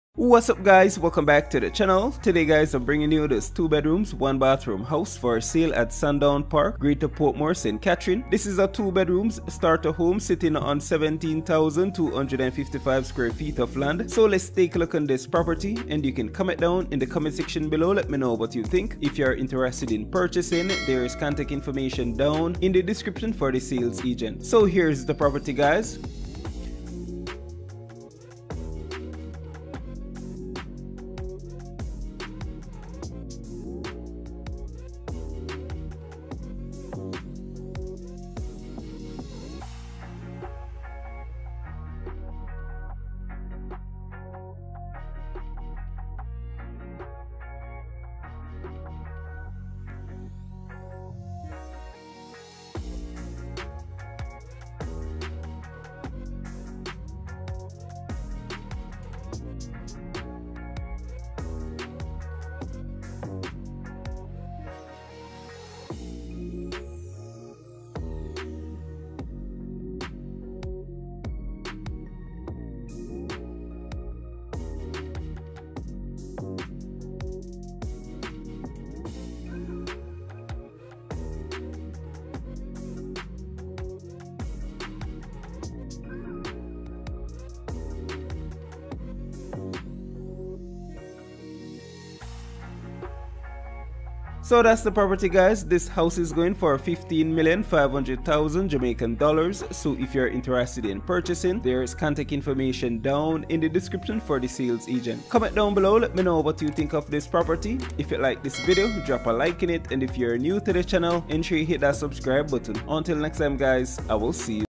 2 Bedrooms 1 Bathroom House For Sale at Sandown Park, Greater Portmore, St Catherine, Jamaica
2 bedroom Starter home.

If interested contact the Sales Agent here:
Petagay Dilworth

JMD $15,500,000